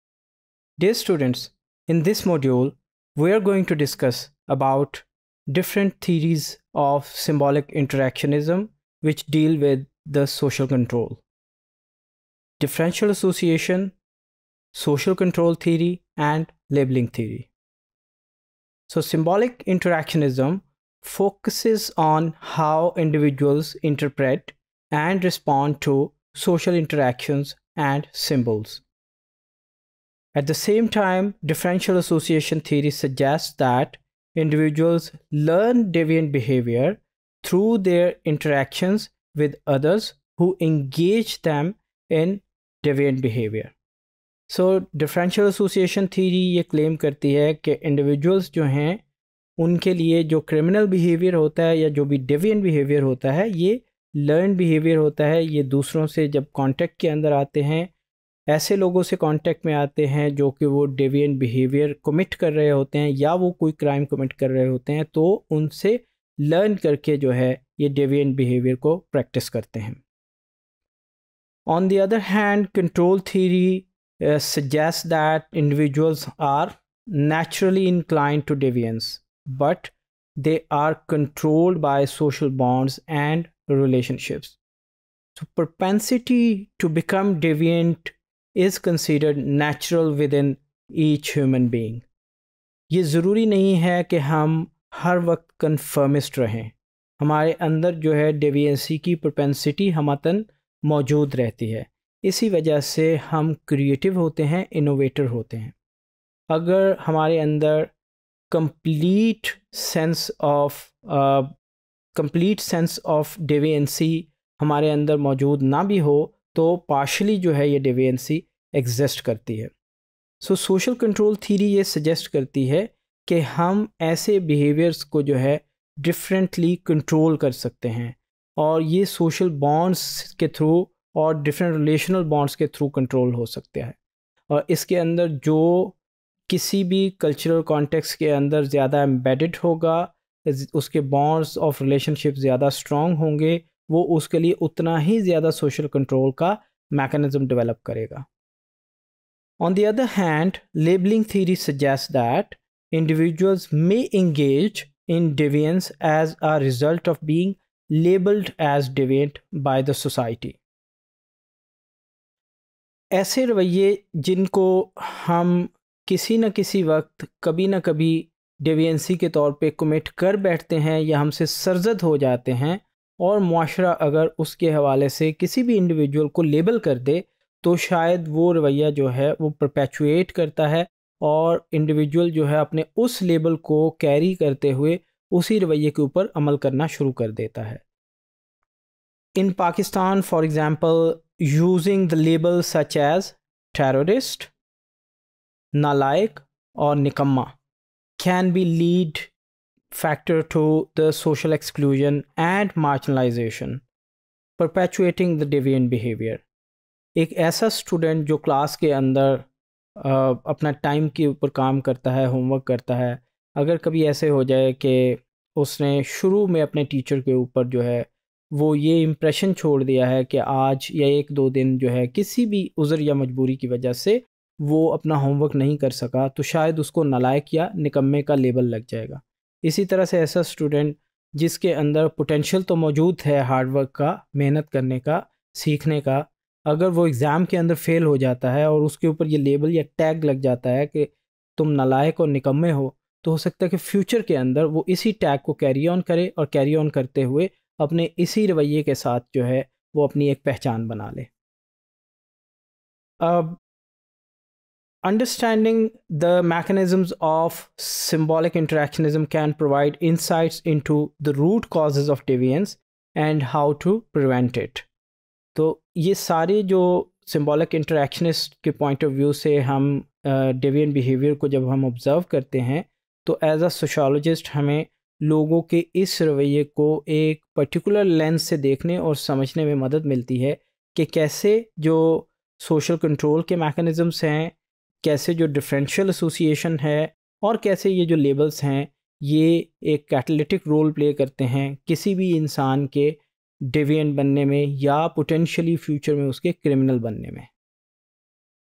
Symbolic Interactionism: Differential Association | Introduction to Sociology | SOC101_Topic093
Topic093: Symbolic Interactionism - Differential Association, Control Theory, Labeling Theory,